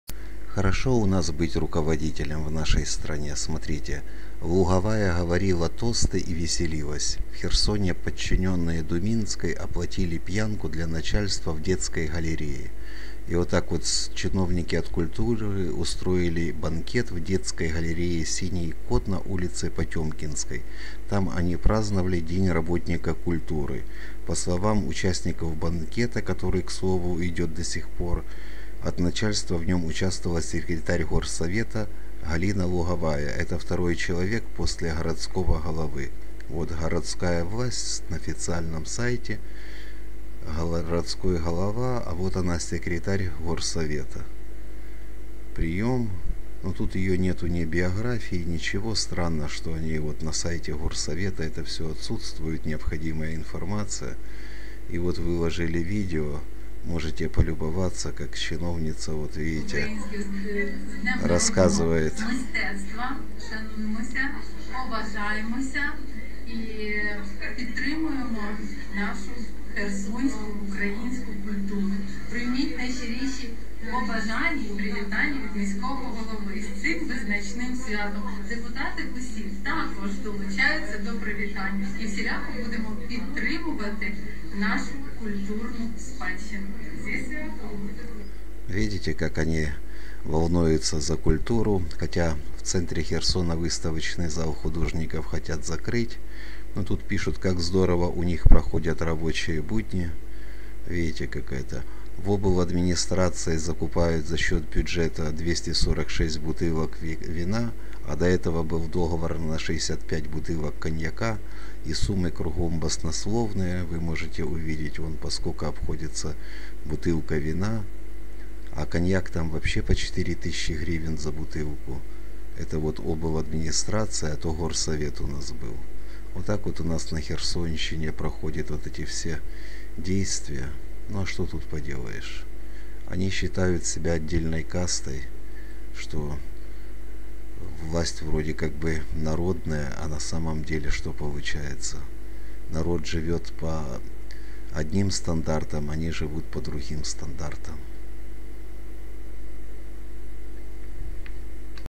Городская власть Херсона веселится. За чей счёт банкет? 9.11.2021 совесть и ответственность
«Луговая приехала с сотрудницей пресс-службы, говорила тосты и веселилась. Наверное не знала, что весь этот богатый банкет был накрыт за наш счет», - рассказал участник банкета, попросивший не называть его, по понятным причинам.
Все дело в том, что деньги на алкоголь и продукты для банкета, из своих премий выделили сами сотрудники управления культуры и руководители учреждений.
«К празднику нам выдали премии, а на следующий день обзвонила бухгалтер и сообщила, что часть денег мы должны отдать на проведение банкета. Так распорядилась Светлана Думинская», - рассказал собеседник
Кроме Луговой мероприятие посетили депутаты из «гуманитарной комиссии» городского совета.